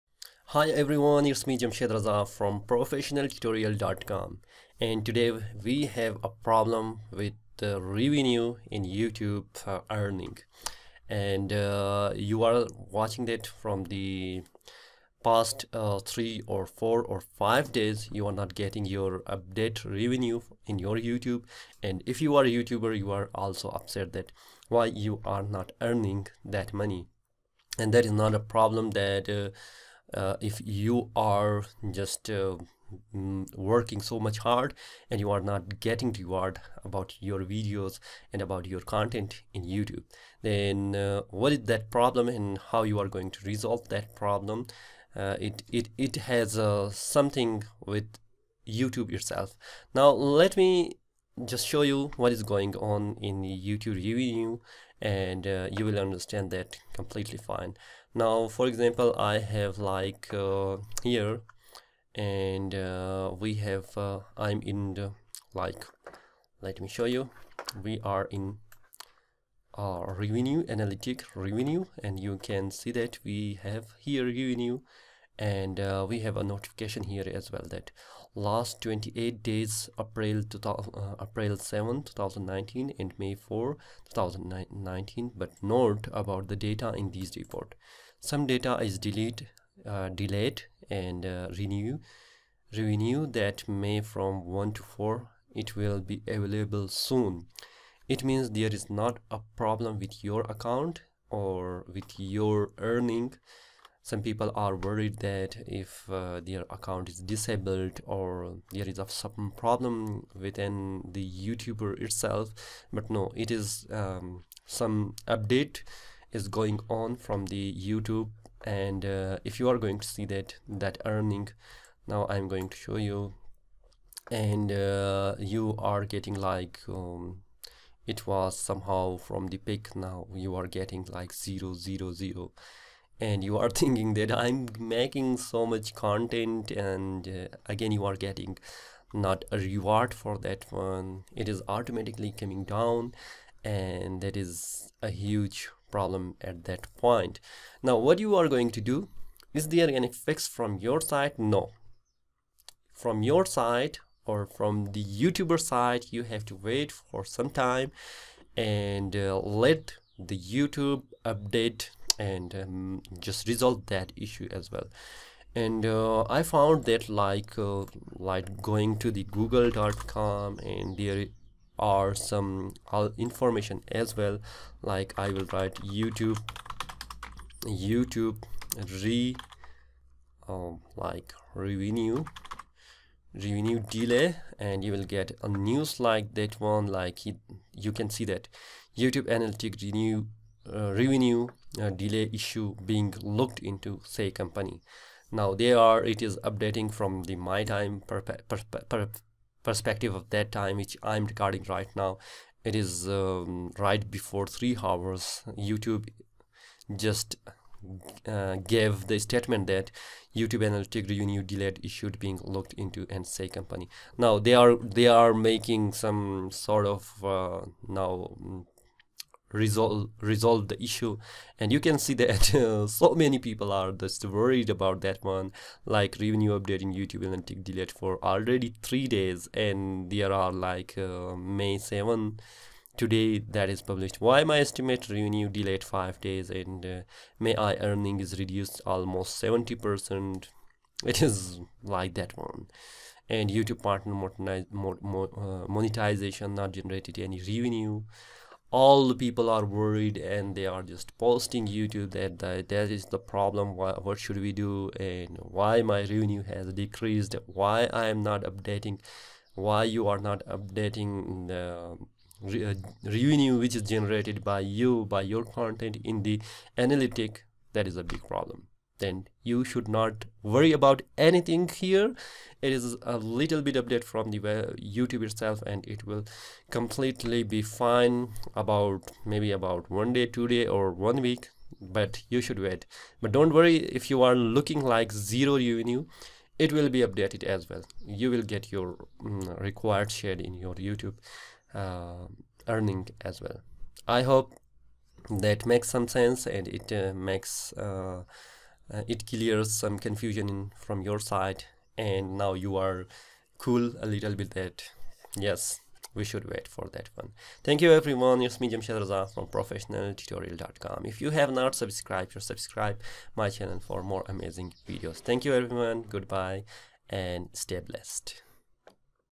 YouTube Estimated Revenue delayed Issue FIXED May 2021
In this video, you will be able to understand how to deal with YouTube Estimated Revenue Delayed Issue. That is not a problem at all. You have to understand simple concept.